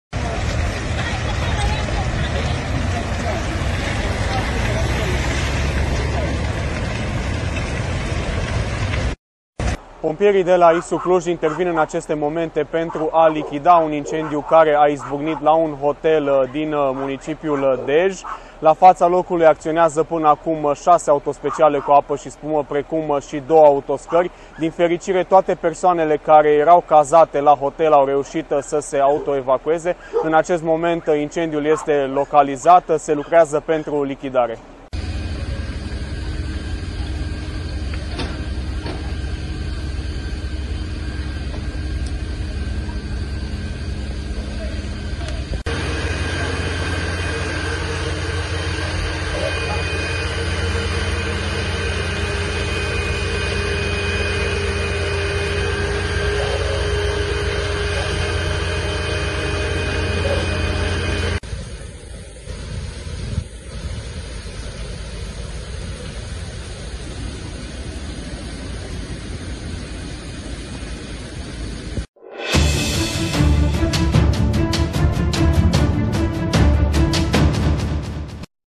Incendiu hotel Dej (Cluj) 5 10 2023
Pompierii au fost alertați să intervină la un incendiu la Dej, la un hotel. vezi mai mult aici